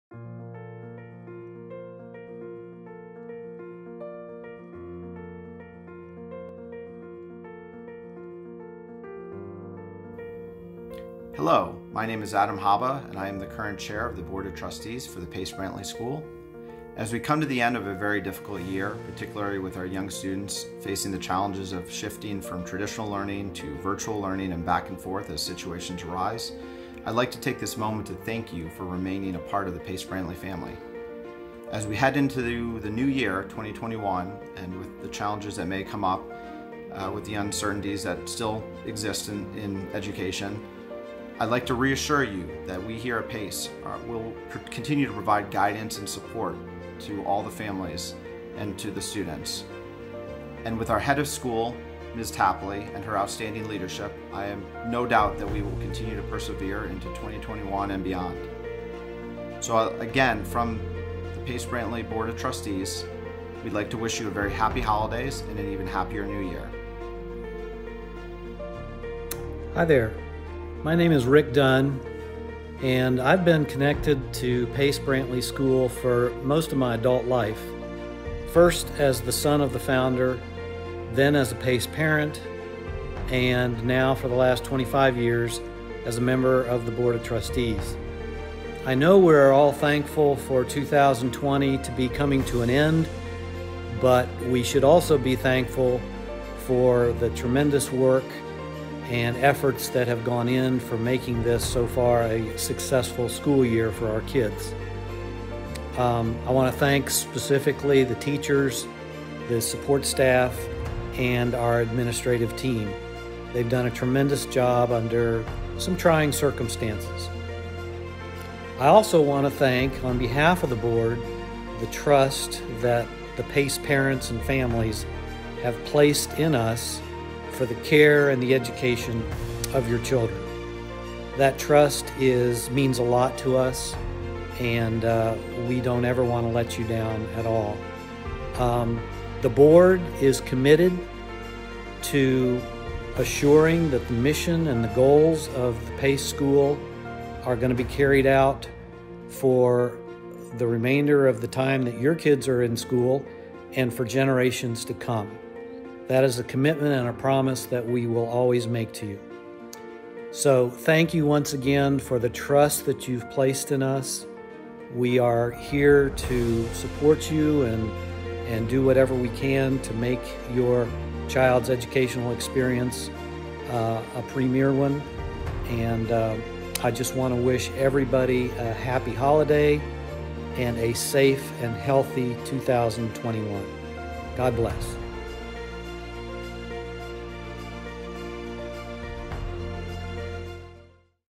Pace Brantley School Board of Trustees Message
The Pace Brantley School Board of Trustees has a special video message for our families.